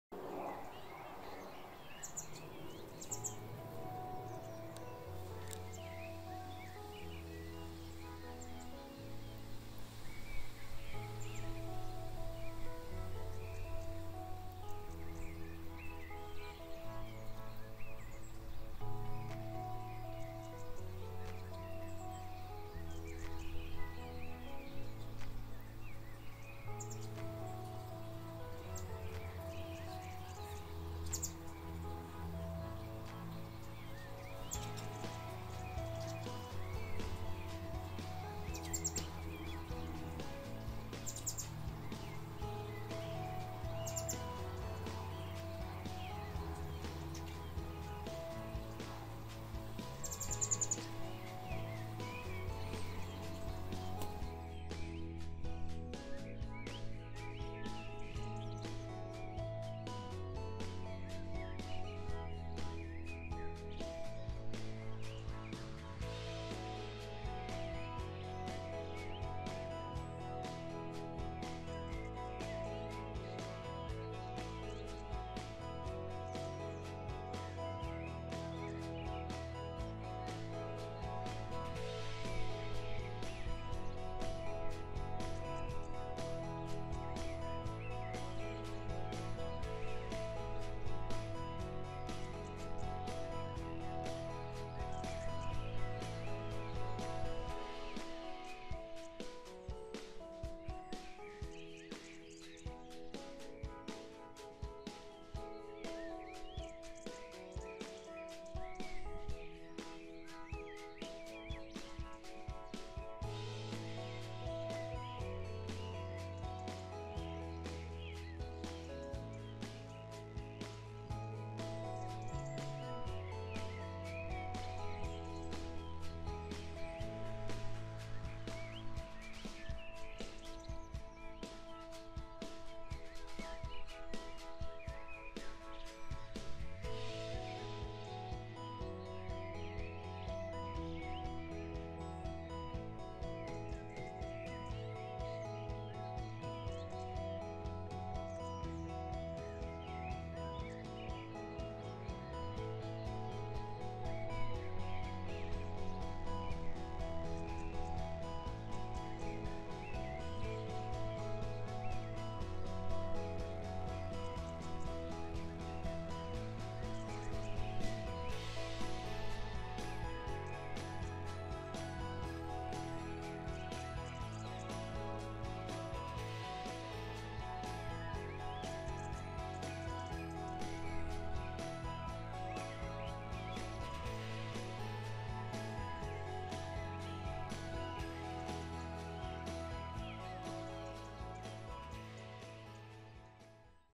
Baby Blackbids | Feeding, Preening, Drinking - Also Featuring Mr & Mrs Blackbird
Some footage of our "pet" wild blackbirds with their chicks. Mrs Blackbird is hand tame and super friendly. The chicks are 9 days since fledging their nest after 15 days in it...